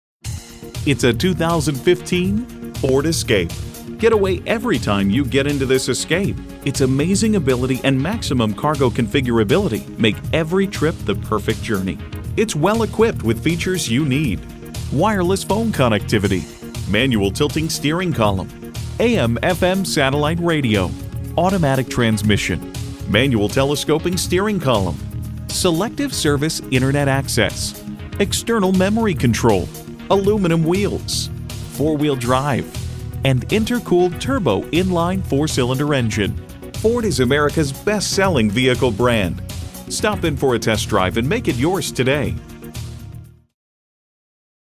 Used 2015 Ford Escape Wausau, WI
Year: 2015
Make: Ford
Model: Escape
Trim: SE
Engine: EcoBoost 2.0L I4 GTDi DOHC Turbocharged VCT
Transmission: 6-Speed Automatic
Color: Silver
Interior: Medium Light Stone
Mileage: 98373
Stock #: 4112FA
VIN: 1FMCU9G98FUA49695

Address:
2727 North 20th Ave